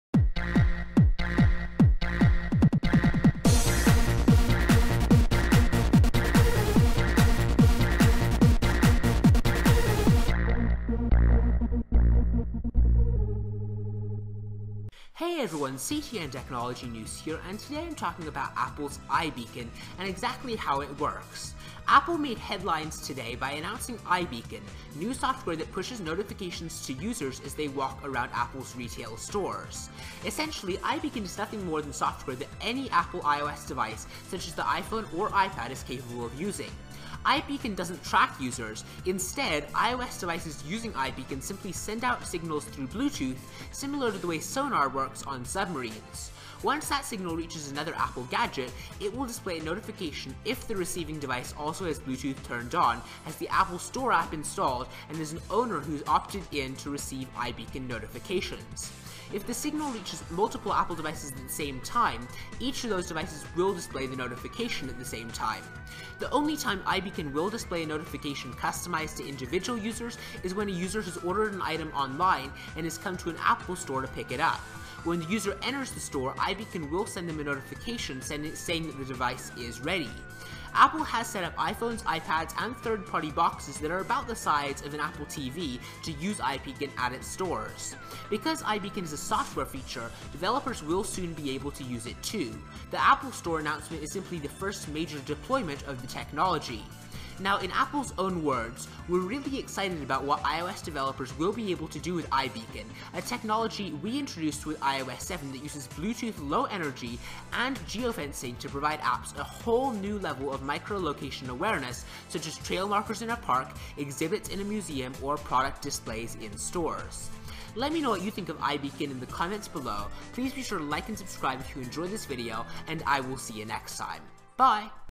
iBeacons: What They Are And How They Work
Apple is making headlines with its new iBeacon software that pushes notifications to users as they walk around the company's retail stores.  But what is it, and how does it work?  Watch this video to find out!